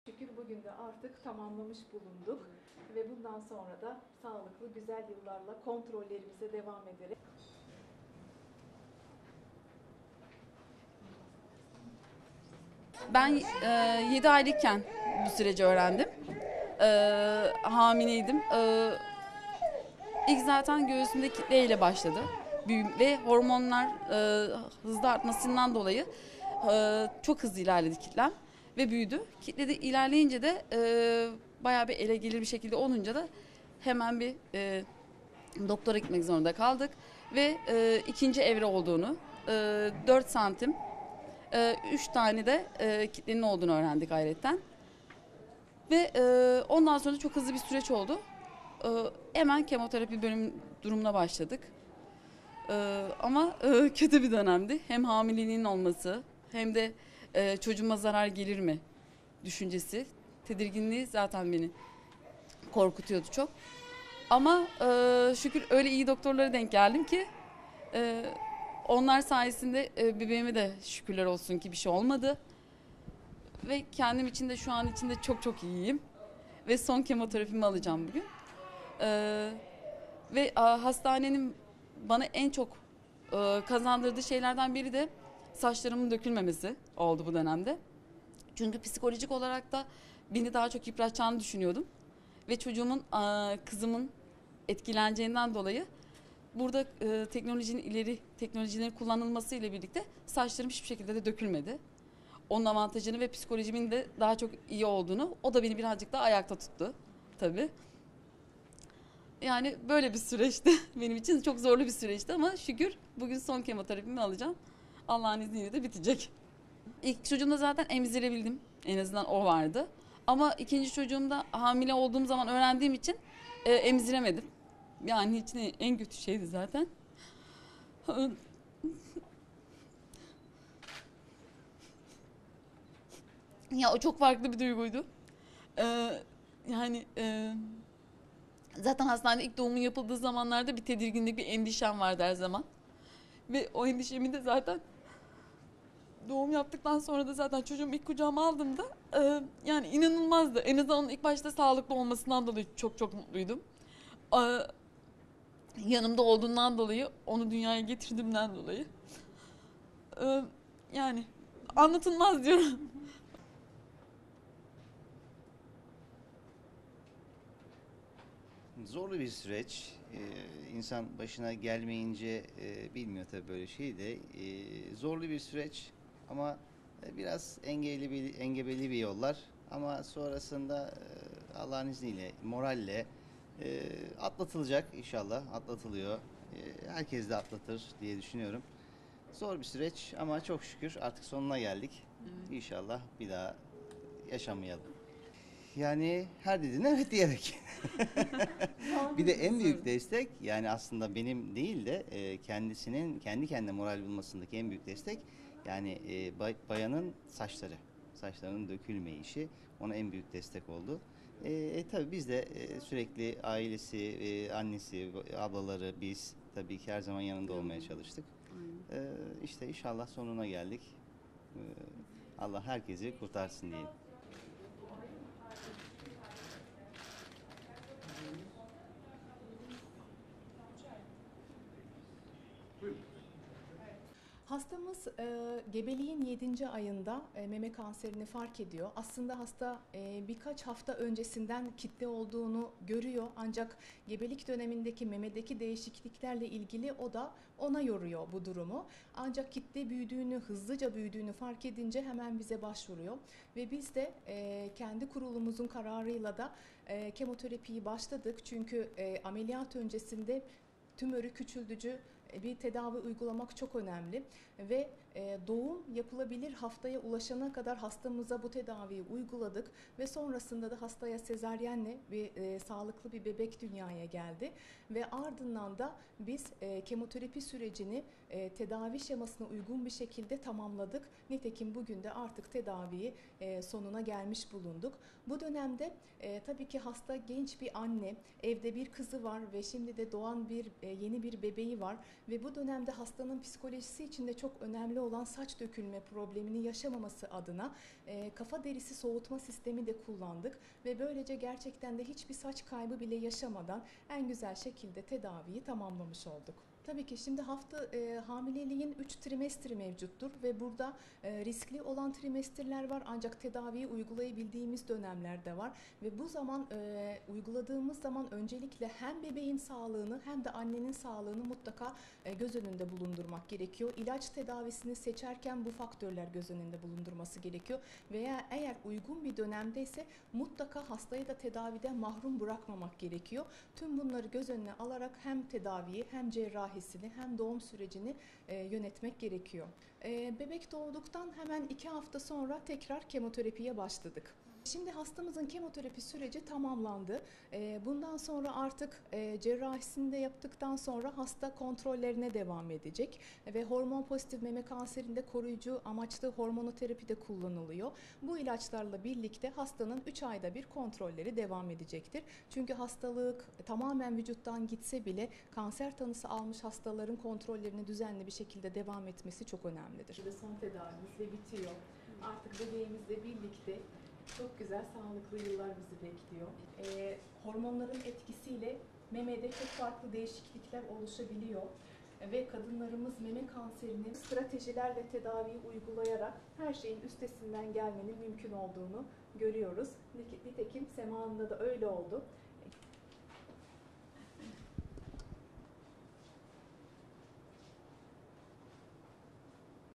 Hem kanseri yendi hem de bebeğini sağlıklı şekilde dünyaya getirdi
İzmir'de, hamileliğinin 7'nci ayında meme kanserine yakalanan Sema Günalay (37) doğumdan 4 ay sonra hastalığı yendi. Kemoterapi almasına karşın, kafa derisi soğutma sistemi sayesinde saç dökülmesi yaşamayan Günalay, "Hamileliğim nedeniyle çocuğuma zarar gelir mi diye çok korktum ama korktuğum gibi olmadı. Bebeğime şükürler olsun bir şey olmadı" dedi.

İzmir'de özel bir nakliye şirketinde çalışan Sema Günalay, hamileliğinin 7'nci ayında göğsünde kitle fark etmesi üzerine, hastaneye gitti ve meme kanserine yakalandığını öğrendi. Düzenli kemoterapi gören ve 4 ay önce bebeği Deniz'i kucağına alan Günalay, Medicana İzmir Hastanesi'nde aldığı son kemoterapinin ardından sağlığına kavuşmanın mutluluğunu yaşıyor. 7 yaşında ilkokul birinci sınıf öğrencisi bir kızı daha bulunduğunu söyleyen Sema Günalay, "İkinci hamileliğim sırasında, 7 aylıkken göğsümde bir kitle oluştu. Hormonlarım nedeniyle kitle hızla büyüdü. İkinci evrede olduğunu ve 4 santim büyüklüğünde 4 ayrı kitlenin olduğunu öğrendik. Hemen kemoterapiye başladık. Hamileliğim nedeniyle çocuğuma zarar gelir mi diye çok korktum ama korktuğum gibi olmadı. Bebeğime şükürler olsun bir şey olmadı. Son kemoterapimi aldım" ifadelerini kullandı.

'DENİZ'İ EMZİREMEMEK BENİM İÇİN EN KÖTÜSÜYDÜ'

Kemoterapi sırasında saçların dökülmemesine çok sevindiğini belirten Günalay, "Saçlarımın dökülmesi psikolojik olarak beni yıpratırdı. Kızımın da etkilenmesinden korktum ama ileri teknolojiler sayesinde hiçbir dökülme olmadı. Bu beni biraz daha ayakta tuttu. İlk kızımı emzirdim ama Deniz'i emzirememek benim için en kötüsüydü. Oğlum doğunca çok farklı bir duygu yaşadım. Endişelerim vardı ama onun sağlıklı biçimde dünyaya gelmesinden dolayı çok mutlu oldum" diye konuştu.

Günalay'ın gayrimenkul sektöründe çalışan eşi Serdal Günalay (40) da tedavi süresince eşine en büyük desteği kendisinin verdiğini belirterek, "Zorlu bir süreci geride bıraktık. İnsan başına gelmeyince anlamıyor. Moral sayesinde atlattık. Çok şükür sonuna geldik. Her dediğine 'Evet' dedim. Saçlarının dökülmeyişi ona en büyük moral oldu ama ailesi olarak da her zaman yanında olmaya çalıştık" dedi.

'DOĞUMDAN 2 HAFTA SONRA KEMOTERAPİ ALDI'

Medicana İzmir Hastanesi Medikal Onkoloji'den Uzm. Dr. Elvina Almurodova da tedavinin başarıyla sonlandığını, bundan sonra cerrahi bir operasyonla memenin alınacağını söyledi. Hastanın düzenli kontrollere devam etmesi gerektiğini belirten Uzm. Dr. Almurodova şöyle devam etti:

"Hastamız göğsündeki kitle büyüyünce bize geldi. Kurul kararıyla kemoterapiye başladık çünkü ameliyat öncesinde tümörü küçültücü bir tedavi uygulamak çok önemli. Doğum yapılabilir haftaya ulaşana kadar bu tedaviyi uyguladık. Sezaryenle doğum yaptı. Kemoterapi sürecini tedavi şemasına uygun şekilde tamamladık. Hasta genç bir anne. Onun psikolojisi için önemli olan saç dökülmemesi adına kafa derisi soğutma sistemini kullandık. Saç kaybı yaşamadan tedaviyi tamamlamış oldu. Hamileliğin riskli dönemleri var. Kemoterapiyi uygularken hem bebeğin hem annenin sağlığını göz önünde bulundurmak gerekiyor. Hasta uygun dönemdeyse tedaviden mahrum bırakmamak gerekiyor. Bebek doğduktan 2 hafta sonra kemoterapiye devam ettik. Kemoterapi tamamlandı. Cerrahisi yapıldıktan sonra hasta kontrollerine devam edecek. Çünkü hastalık tamamen vücuttan gitse bile kanser tanısı almış hastaların kontrollerine düzenli şekilde devam etmeleri çok önemlidir."